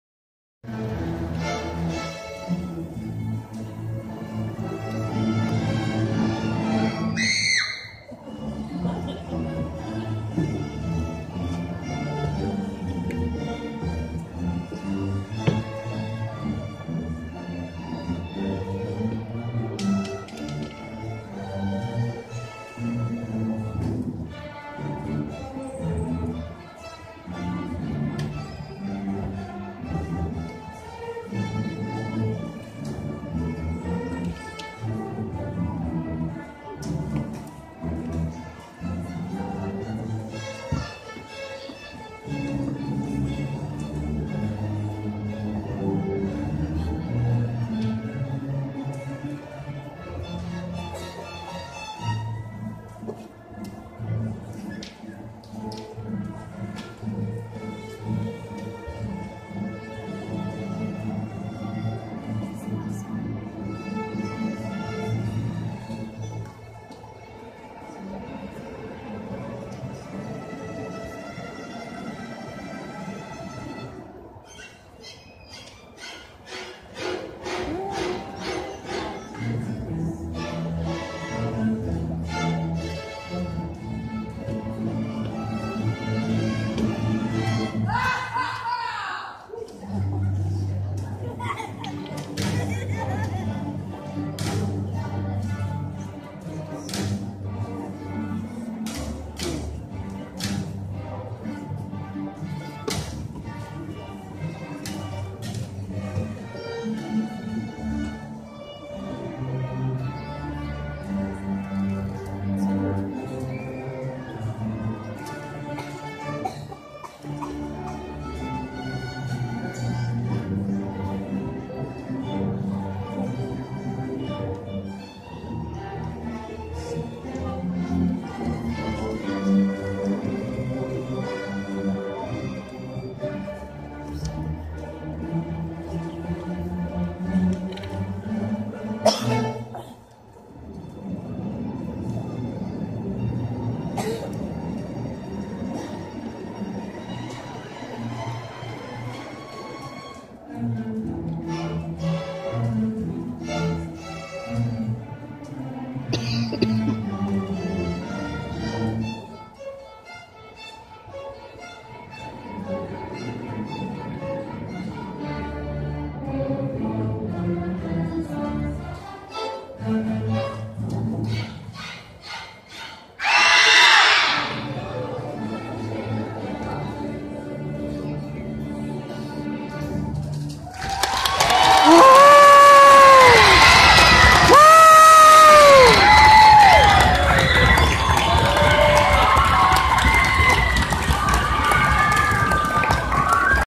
Jr's Halloween Glow-Stick Concert: Part 4 (Taking Place at Westport Middle School in Louisville)
4th part of Jr's Concert (Jr playing Cello) @jcpsschools9408 @BenSolleeKY / SpookyGMom / Sarabeth Bah